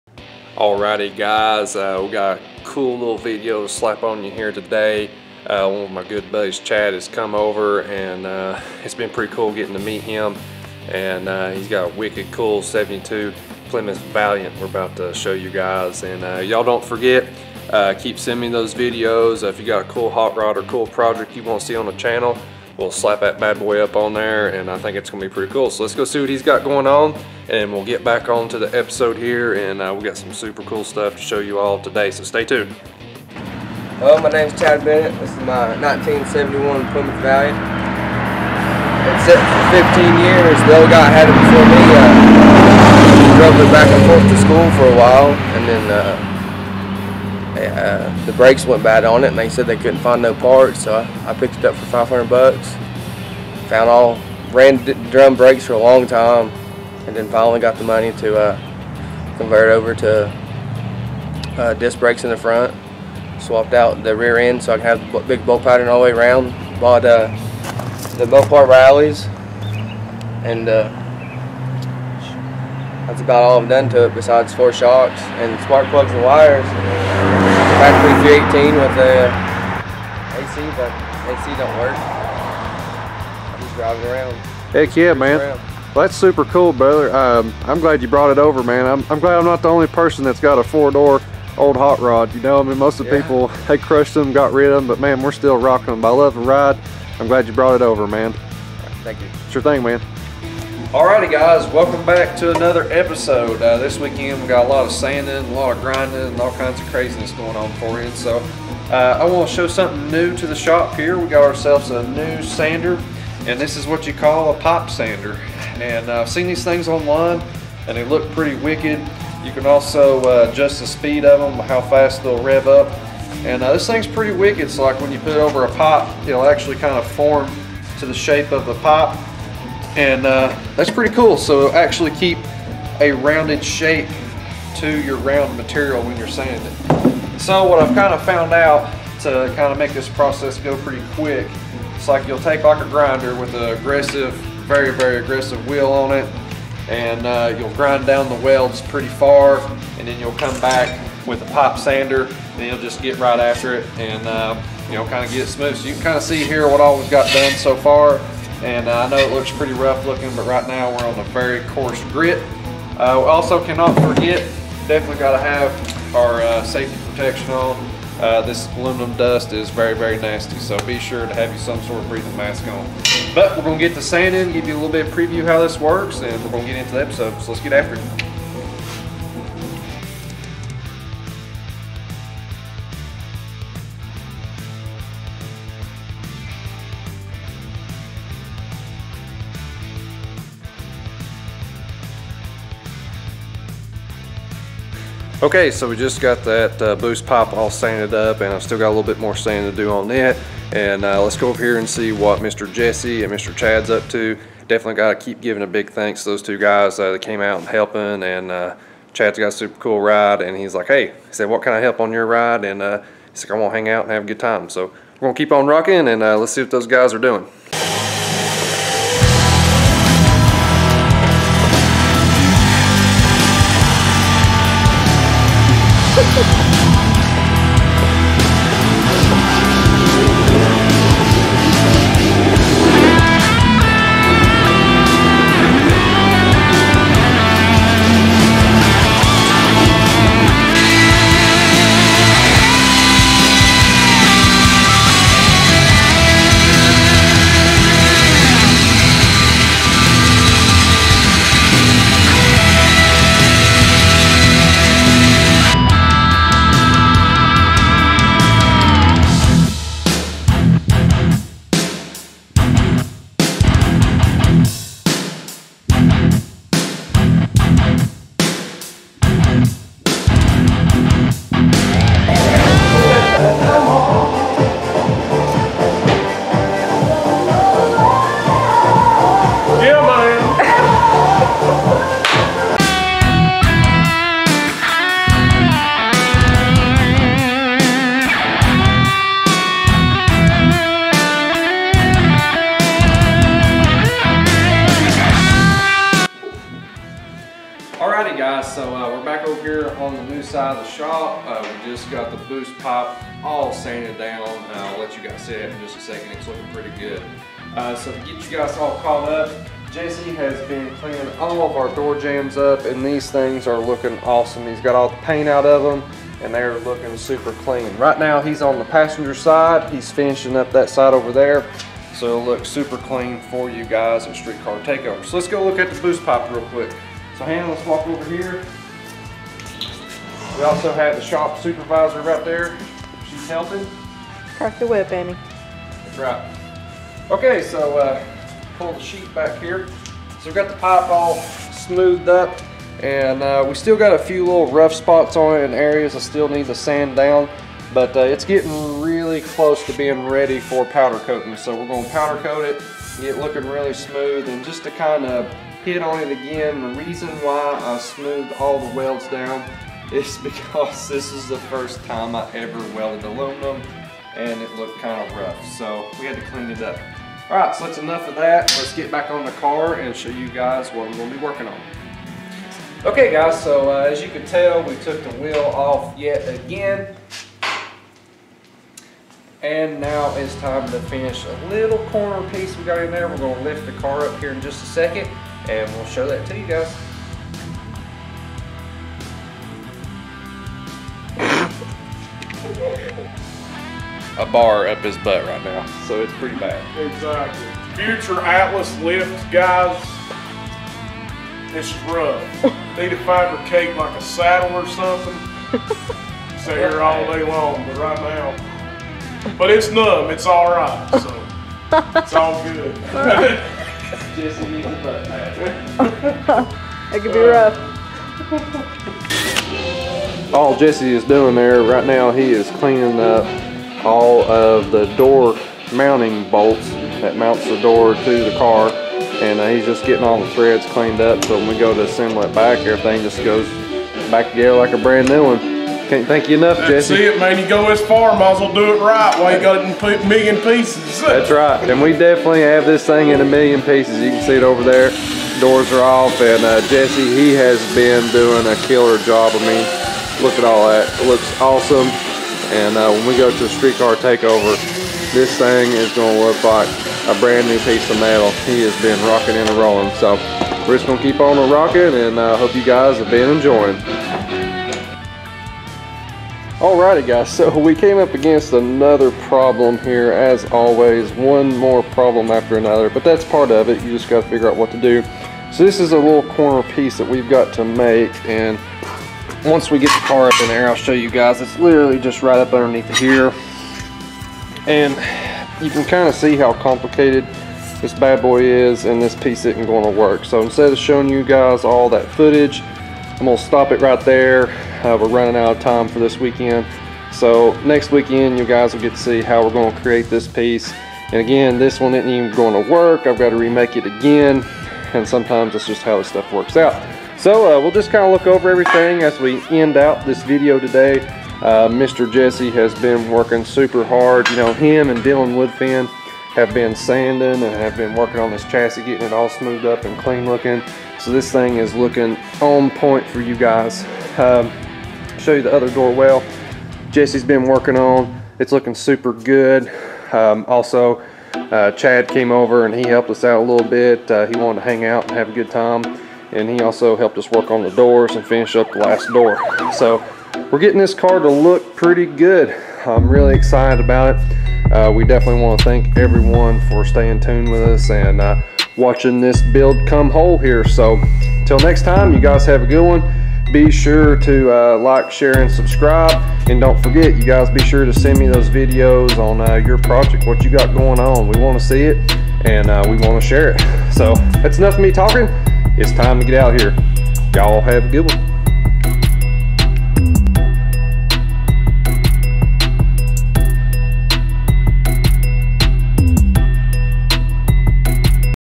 1972 Plymouth Valiant and a wicked cool new pipe sander. (GTO BUILD EP30)
Super cool 72 valaint feature in this weeks episode. We are still hammering away on details getting the GTO ready.

link for music:  chase